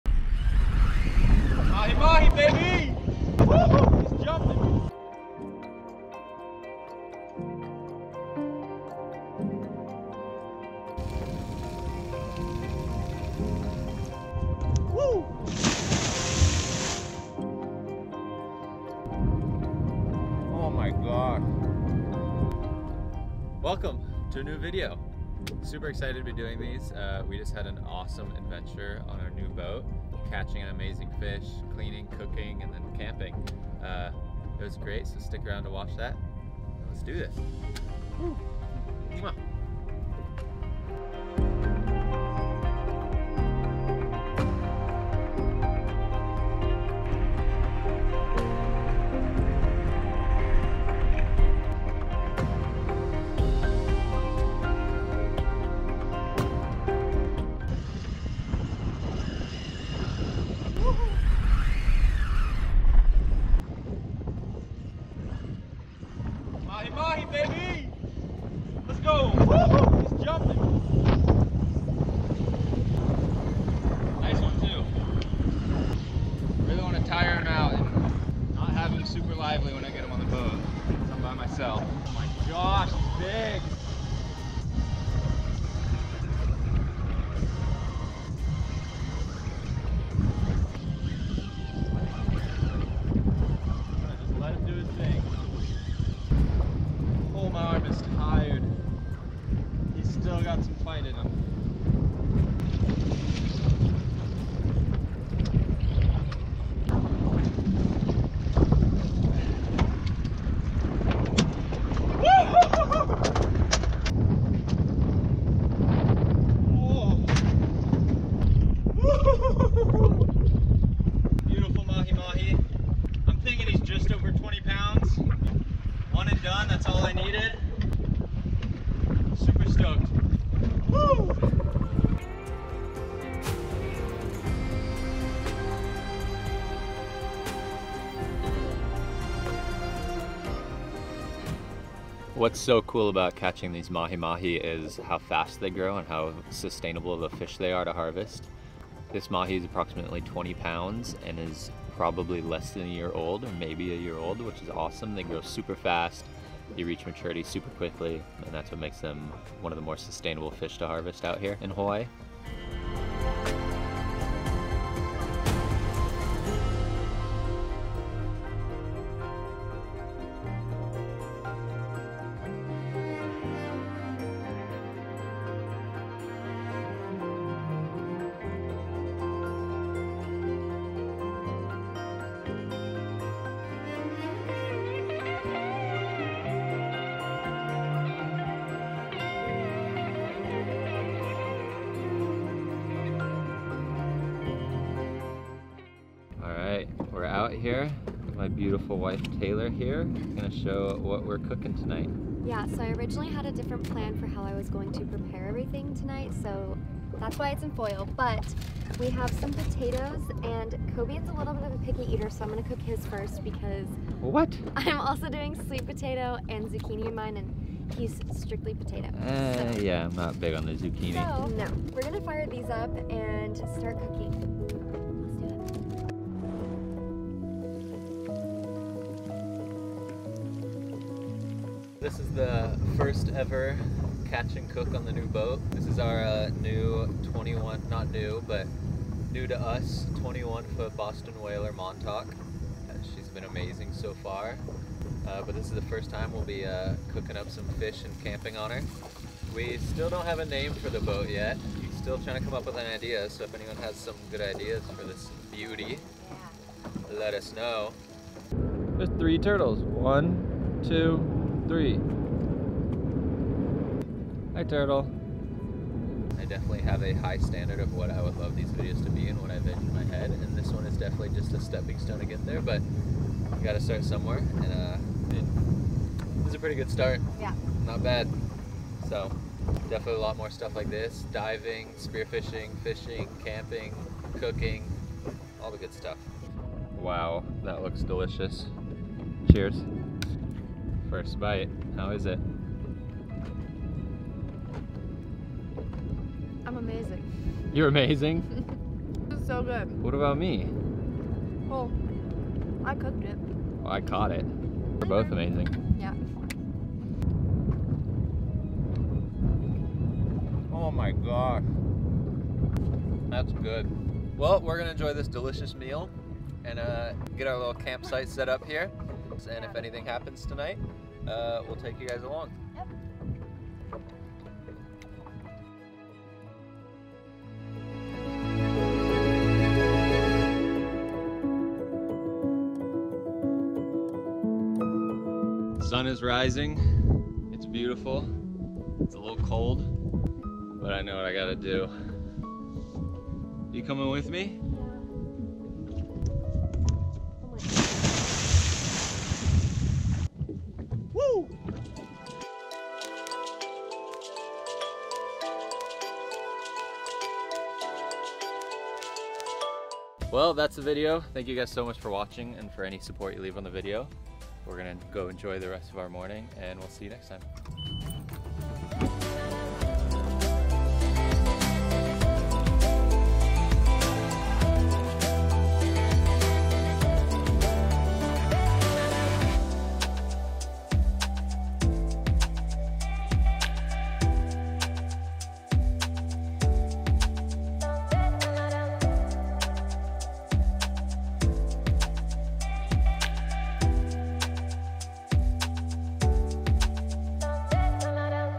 Mahi Mahi Catch & Cook & Camp on our NEW BOAT!!!
On this adventure, I catch my personal best Mahi Mahi, and my wife and I cook it up while camping on our new boat!!